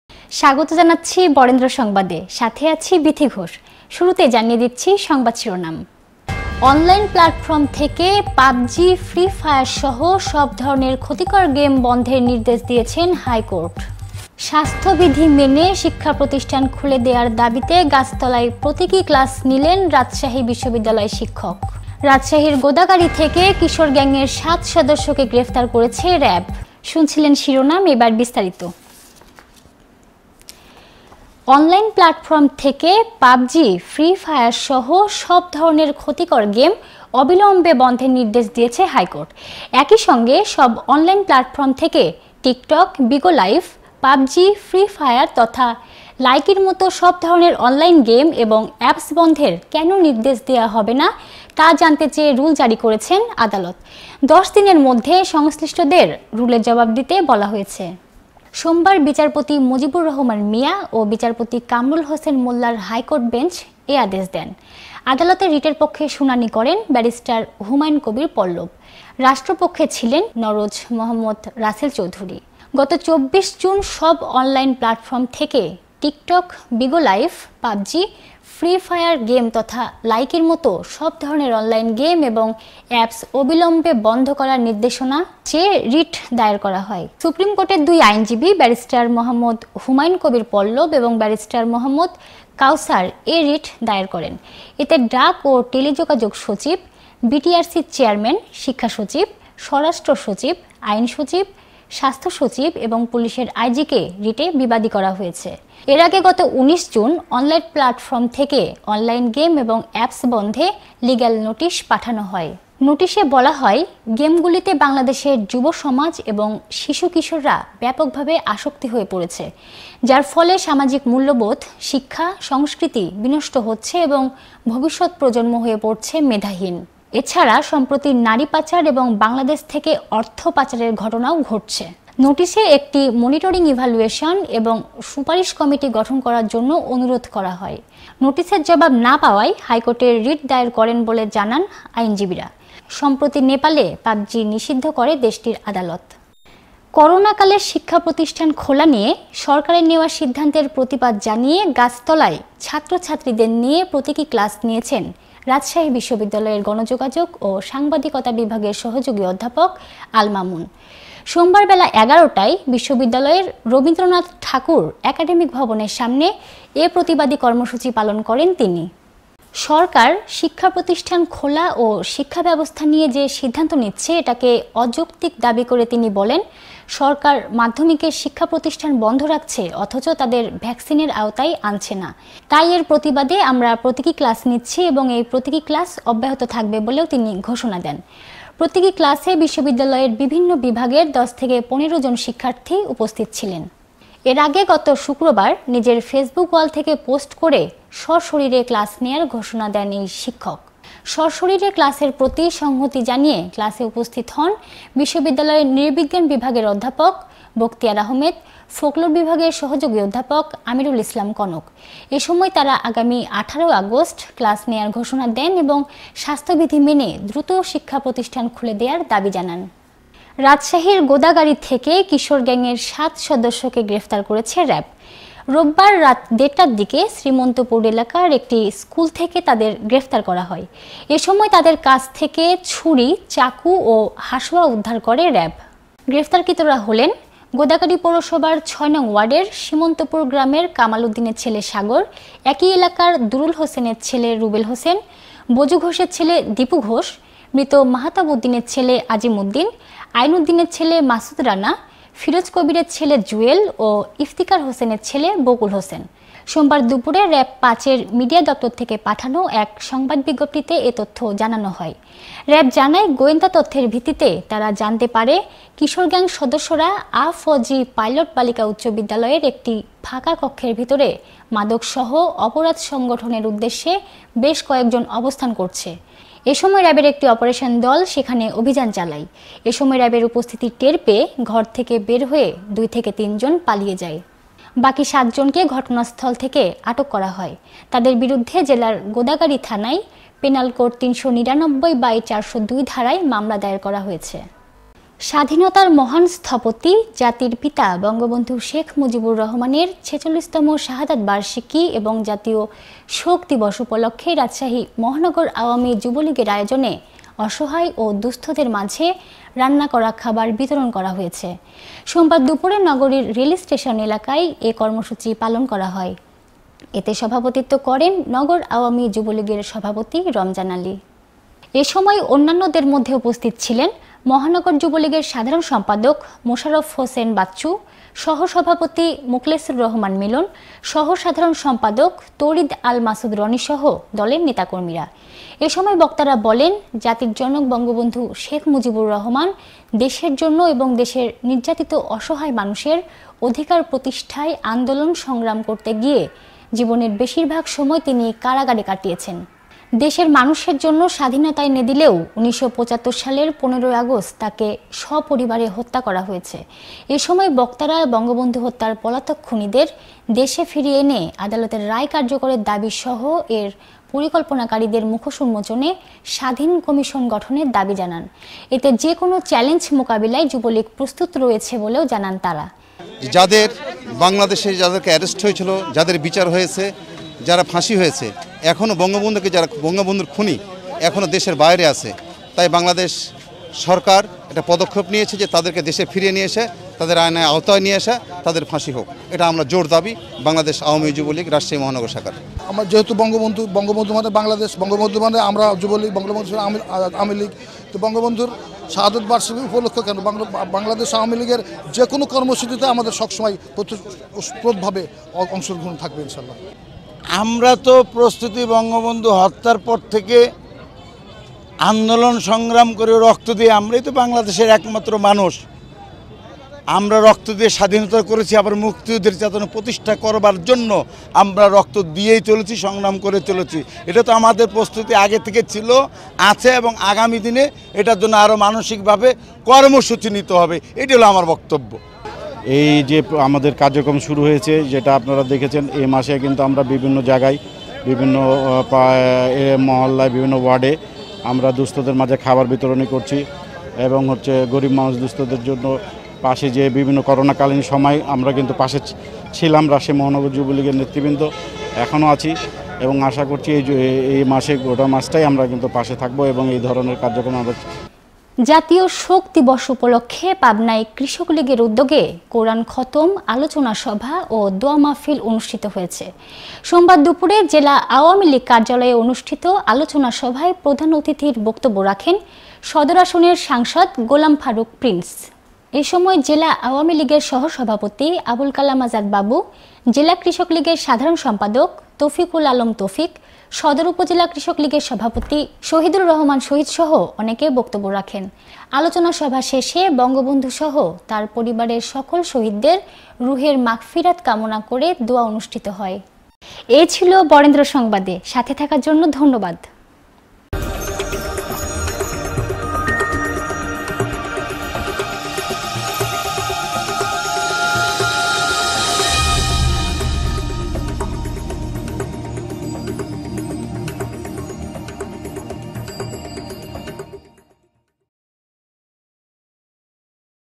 রাজশাহীর গোদাগাড়ী থেকে কিশোর গ্যাংয়ের সাত সদস্যকে গ্রেপ্তার করেছে র‌্যাব
রাজশাহীর গোদাগাড়ী থেকে কিশোর গ্যাংয়ের সাত সদস্যকে গ্রেপ্তার করেছে র‌্যাব | RAB has arrested seven members of a teenage gang from Godagari in Rajshahi

রাজশাহীর গোদাগাড়ী থেকে কিশোর গ্যাংয়ের সাত সদস্যকে গ্রেপ্তার করেছে র‌্যাব। রোববার রাত দেড়টার দিকে শ্রীমন্তীপুর এলাকার একটি স্কুল থেকে তাদের গ্রেপ্তার করা হয়। এ সময় তাদের কাছ থেকে তাদের কাছ থেকে ছুরি, চাকু ও হাসুয়া উদ্ধার করে র‌্যাব।
গ্রেপ্তারকৃতরা হলেন, গোদাগাড়ী পৌরসভার ৬ নং ওয়ার্ডের শ্রীমন্তপুর গ্রামের কামাল উদ্দিনের ছেলে সাগর, একি এলাকার দুরুল হোসেনের ছেলে রুবেল হোসেন , বজু ঘোষের ছেলে দিপু ঘোষ , মৃত মাহাতাব উদ্দিনের ছেলে আজিম উদ্দিন, আইনুদ্দিনের ছেলে মাসুদ রানা, ফিরোজ কবিরের ছেলে জুয়েল ও ইফতিখার হোসেনের ছেলে বকুল হোসেন । সোমবার দুপুরে র‌্যাব-৫ এর মিডিয়া দপ্তর থেকে পাঠানো এক সংবাদ বিজ্ঞপ্তিতে এ তথ্য জানানো হয়, র‌্যাব জানায় গোয়েন্দা তথ্যের ভিত্তিতে তারা জানতে পারে কিশোর গ্যাং সদস্যরা আ.ফ.জি পাইলট বালিকা উচ্চ বিদ্যালয়ের একটি ফাঁকা কক্ষের ভিতরে মাদকসহ অপরাধ সংঘটনের উদ্দেশ্যে বেশ কয়েক জন অবস্থান করছে। এ সময় র‌্যাবের একটি অপারেশন দল সেখানে অভিযান চালায়। এ সময় র‌্যাবের উপস্থিতি টের পেয়ে ঘর থেকে বের হয়ে ২/৩ জন পালিয়ে যায়। বাকি সাতজনকে ঘটনাস্থল থেকে আটক করা হয়। তাদের বিরুদ্ধে জেলার গোদাগাড়ী থানায় পেনাল কোড ৩৯৯/৪০২ ধারায় মামলা দায়ের করা হয়েছে।

RAB has arrested seven members of a teenage gang from Godagari in Rajshahi
RAB has arrested seven members of a teenage gang from Godagari in Rajshahi. They were arrested from a school in Srimantipur area around 1.30 pm on Sunday. At that time, RAB recovered knives, knives and machete from them.
The arrested are Sagar, son of Kamal Uddin of Srimantapur village in Ward 6 of Godagari Municipality, Rubel Hossain, son of Durul Hossain of the same area, Dipu Ghosh, son of Baju Ghosh, Azim Uddin, son of late Mahatab Uddin, Masud Rana, son of Ainuddin and Jewel Hossain, son of Firoz Kabir. Hossain. According to a press release issued by RAB-5's media office on Monday afternoon, RAB said that based on intelligence information, they came to know that the members of the juvenile gang were located inside an empty room of AFG Pilot Girls' High School. Doing. At that time an operation team of RAB conducted an operation there. At this time, sensing the presence of the RAB, 2/3 people left the house and fled. The other seven were arrested from the spot. A case has been filed against them under section 399/402 of the Penal Code at Godagari police station.